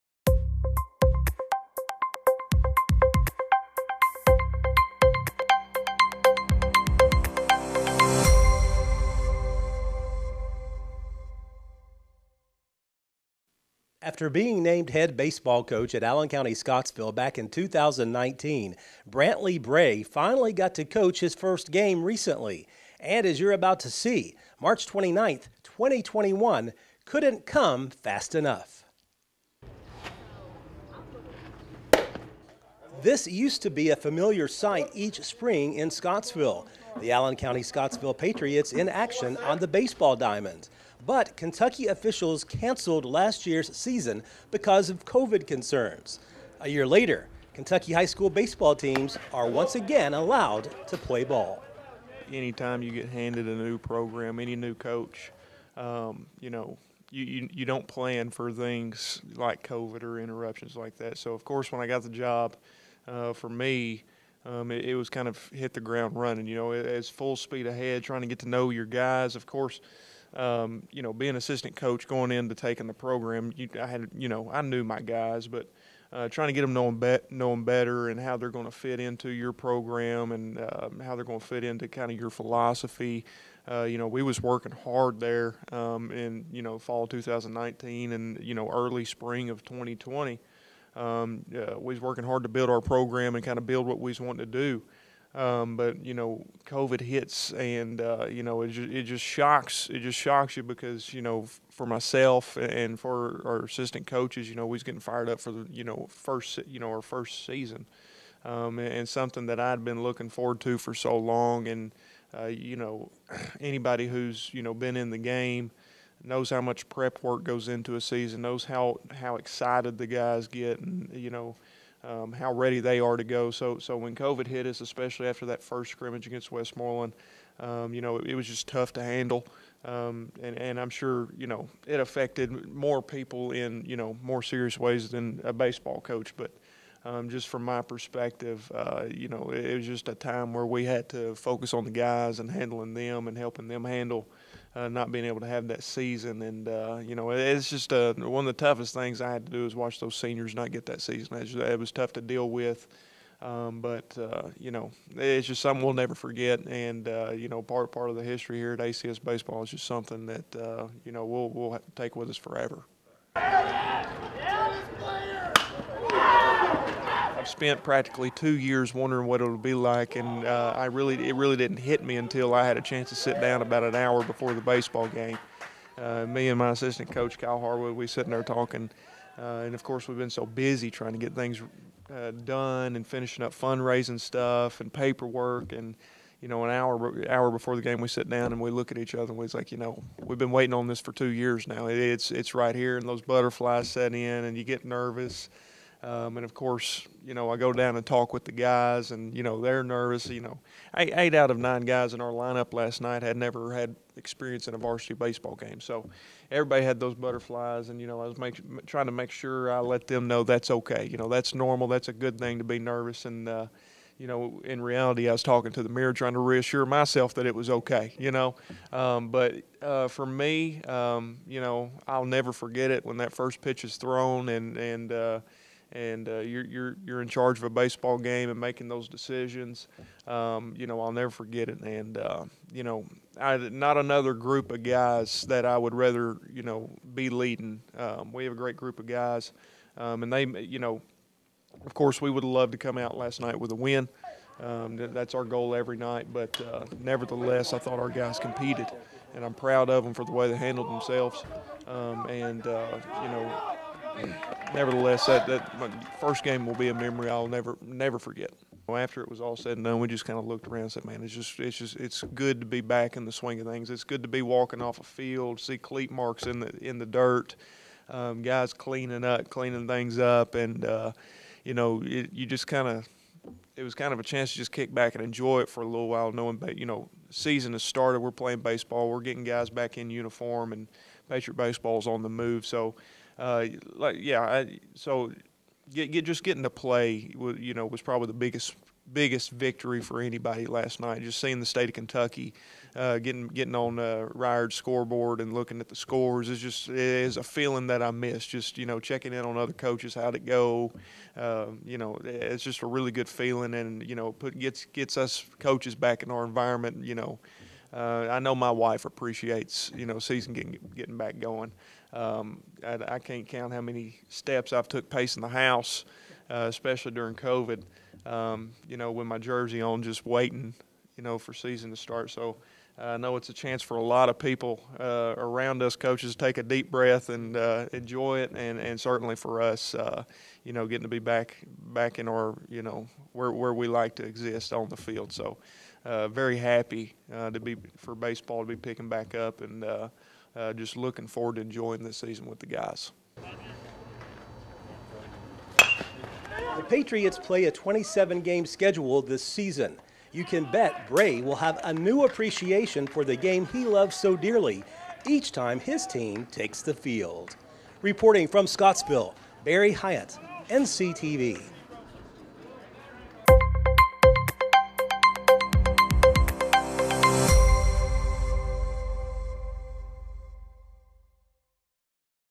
BrantlyBrayFirstACSGame
After waiting for about a year and a half, Allen County Scottsville head baseball coach Brantly Bray finally gets to take the field as the Patriots new coach on March 29, 2021!